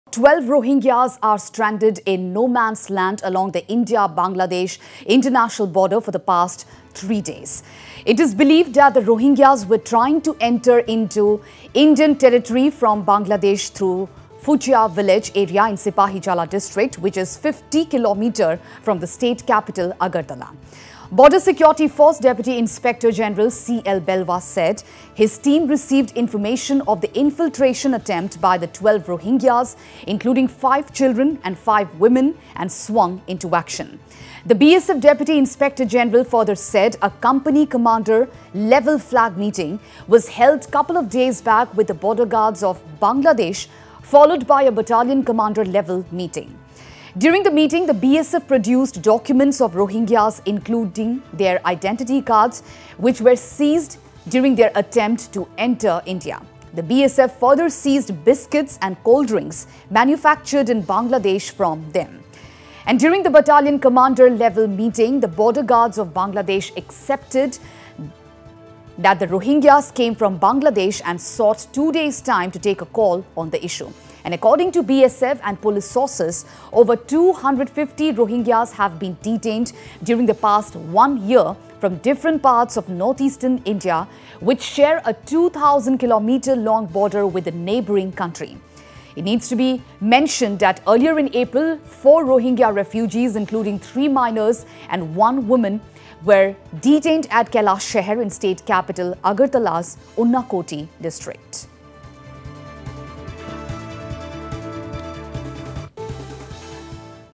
12 Rohingyas stranded in Indo-Bangladesh border in Tripura
12 Rohingyas are stranded in 'no man’s land’ along the India-Bangladesh international border for the past 3 days.
It is believed that the Rohingyas were trying to enter into Indian Territory from Bangladesh through Futia village area in Sipahijala district, which is 50 km from the State capital Agartala.
Border Security Force Deputy Inspector General CL Belwa said his team received information of the infiltration attempt by the 12 Rohingyas including 5 children and 5 women and swung into action.